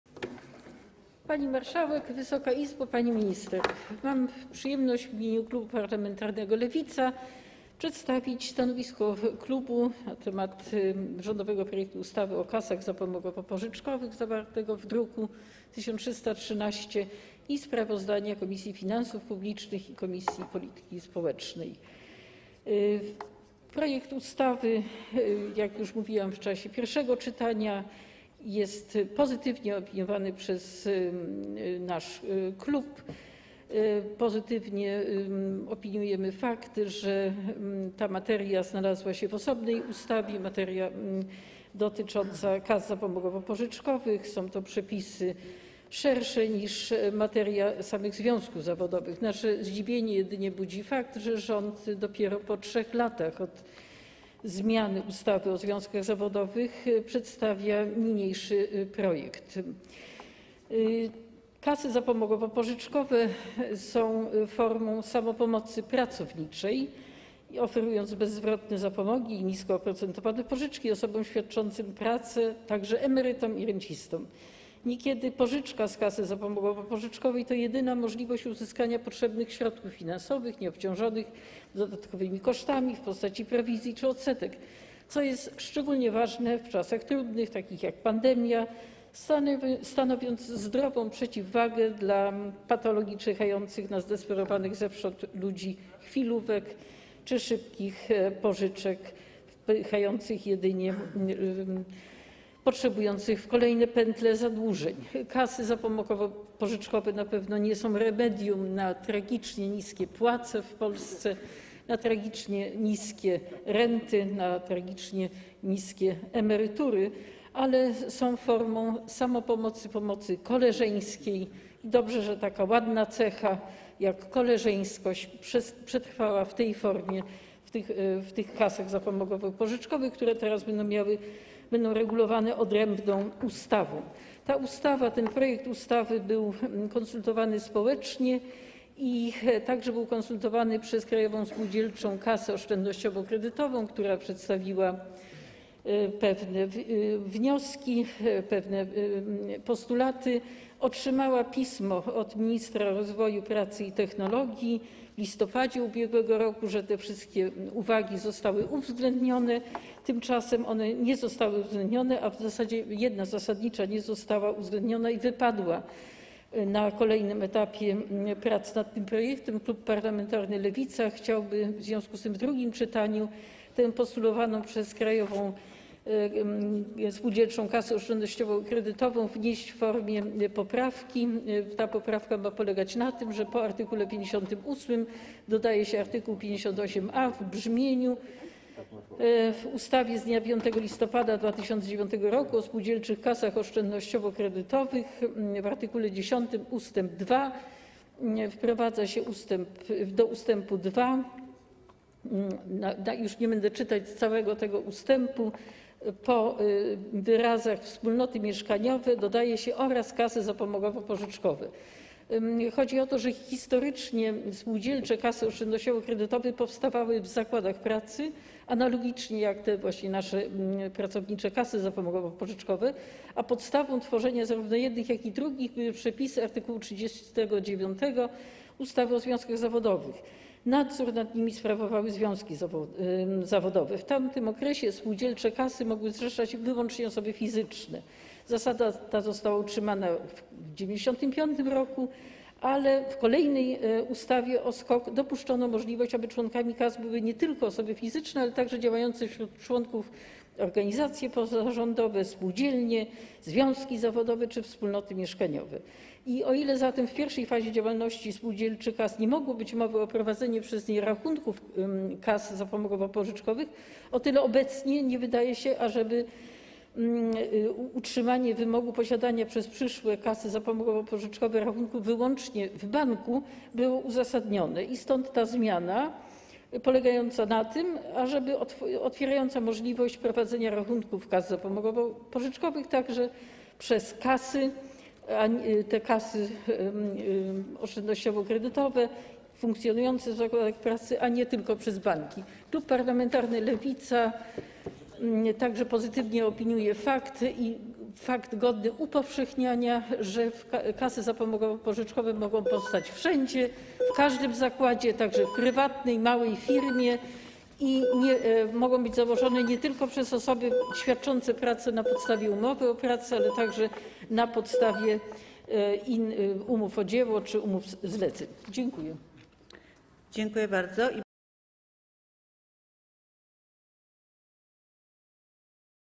Katarzyna Kretkowska o kasach zapomogowo-pożyczkowych
Popierasz? Udostępnij! Katarzyna Kretkowska - wystąpienie z 21 lipca 2021 r.

Sprawozdanie Komisji o rządowym projekcie ustawy o kasach zapomogowo-pożyczkowych